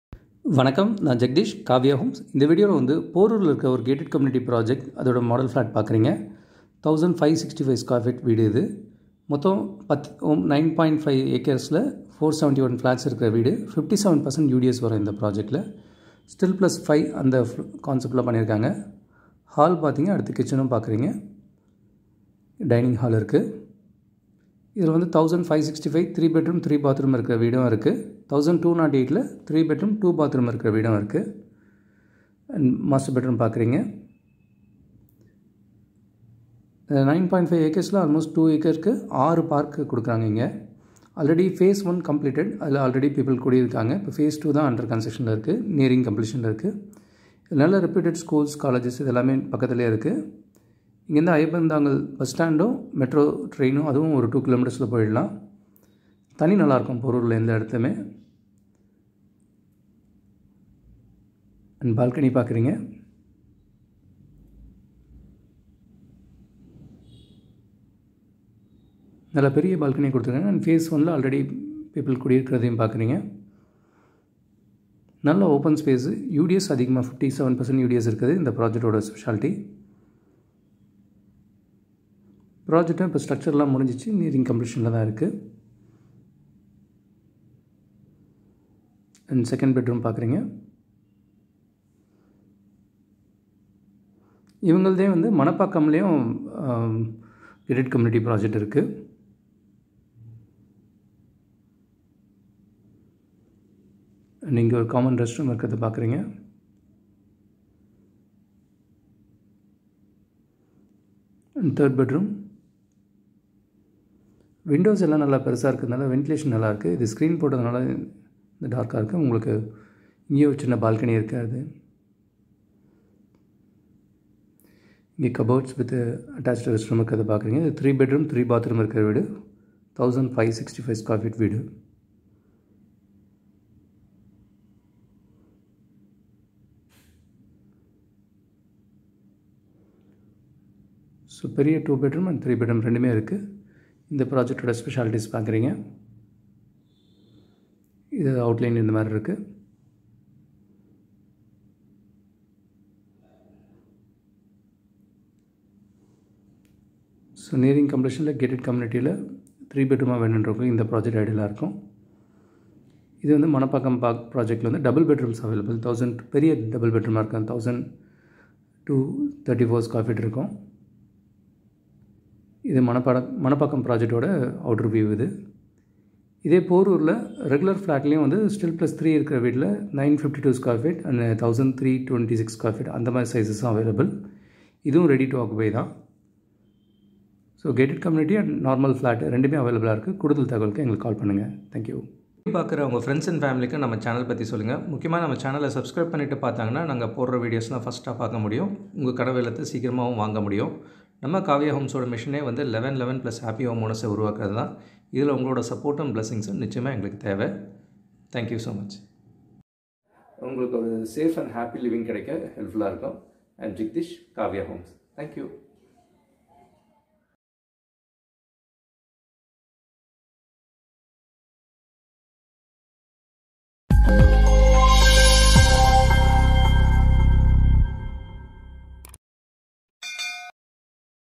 Porur ready built 3bhk flats for sale! kaaviya homes
Ready built 3bhk flats with gated community amenities like big parks, club house, car park, lift, cctv, play area, security, good ventilation, 57% UDS, abundant water, very close to reputed schools!
Sizes available, flats  – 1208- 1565 sft, budget – Rs.90 -1.15cr

For more details, kindly reply me at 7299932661/ 2
Jagadeesh
Our Kaaviya homes mission is to create 1111+ happy home owners!